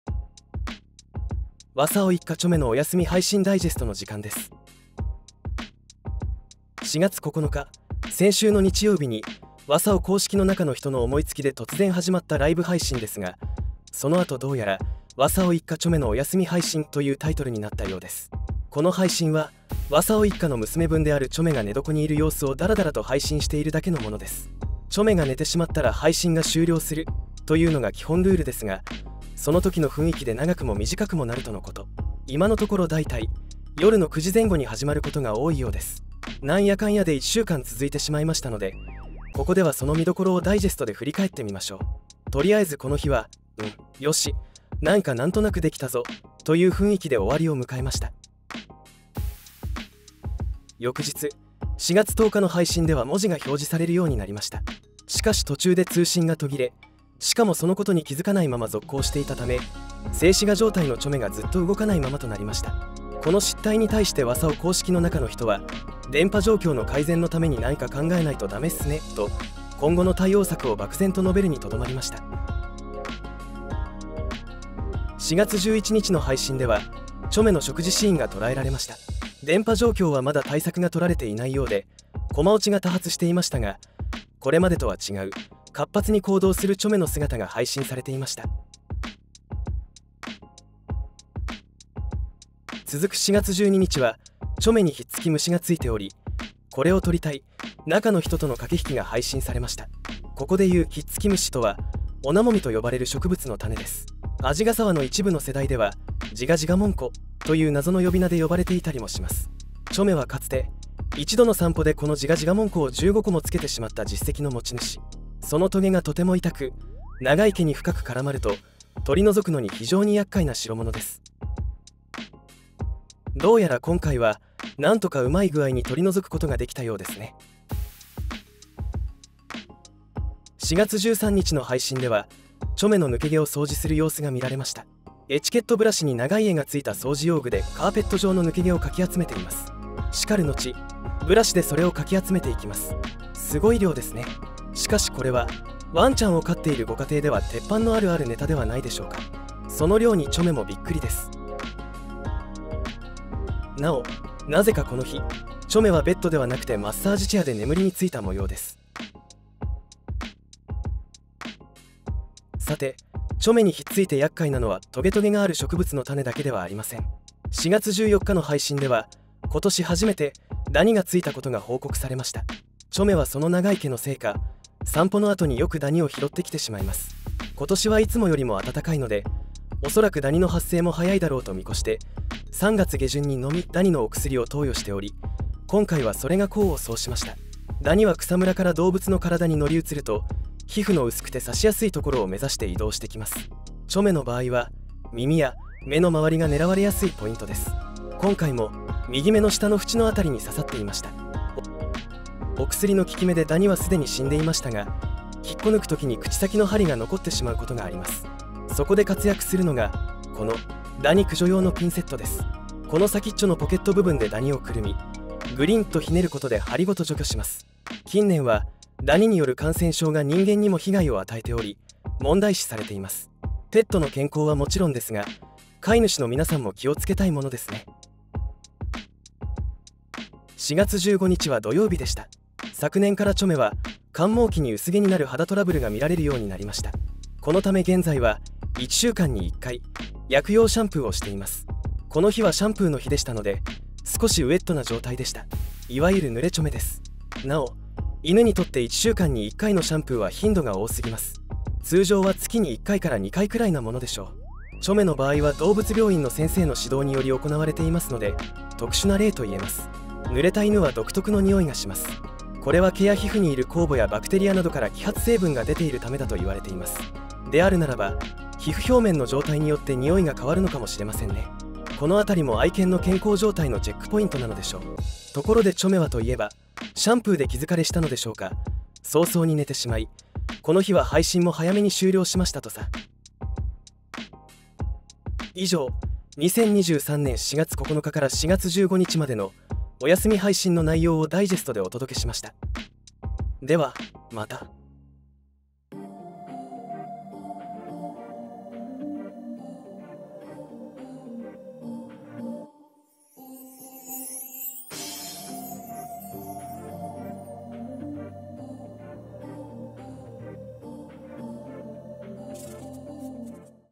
わさおの娘分ちょめ配信ダイジェスト 23/04/09－04/15
わさお一家ちょめのおやすみ配信 一週間分のダイジェストです。
5分くらいに短縮してます。